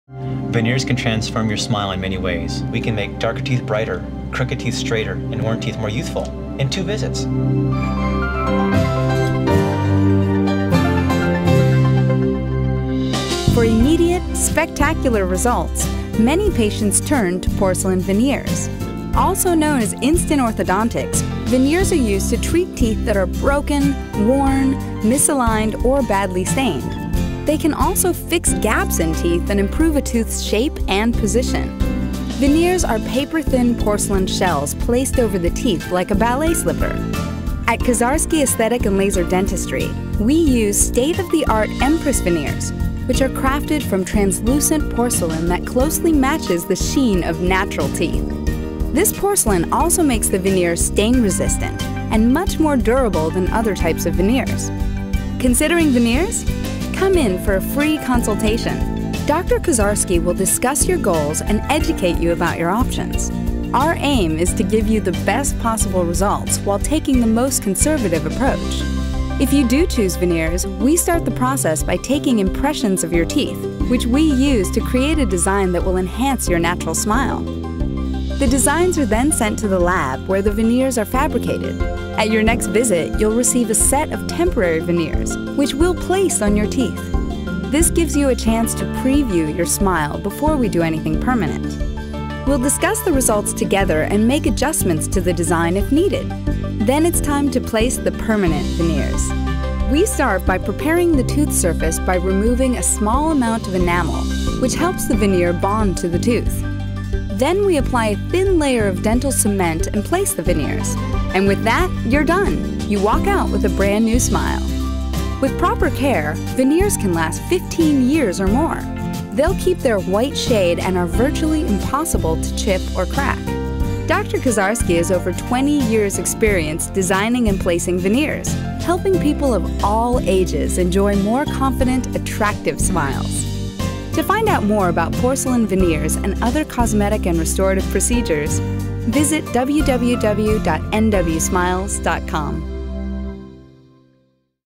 Porcelain Veneers
- The Porcelain Veneers video shows how these shell-like coverings can make broken, misaligned, or badly stained teeth look perfect. Youll see how versatile veneers are, and how they can treat a wide variety of cosmetic issues.